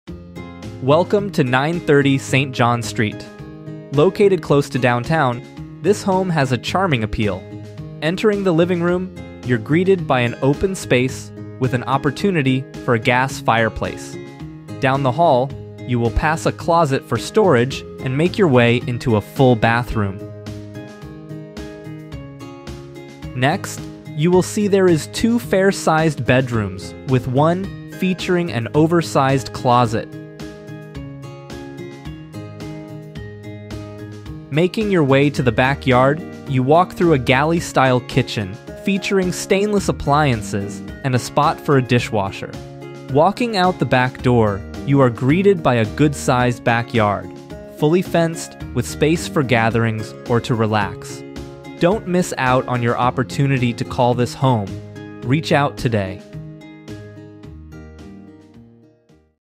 Home For Sale | 930 St John Street, Walla Walla
Discover the charm of this 756 sq ft home in Walla Walla's vibrant heart. Inside, the new vinyl plank flooring adds a fresh, modern touch, creating a welcoming atmosphere that invites you to relax and stay awhile. The kitchen, outfitted with essential appliances, is ready to host your culinary adventures, while the handy in-unit washer and dryer simplify laundry days. Enjoy year-round comfort with the efficient ductless mini-split system, and envision leisurely afternoons in the spacious backyard. Centrally located and move-in ready, 930 St John Street is the perfect canvas for your new beginning.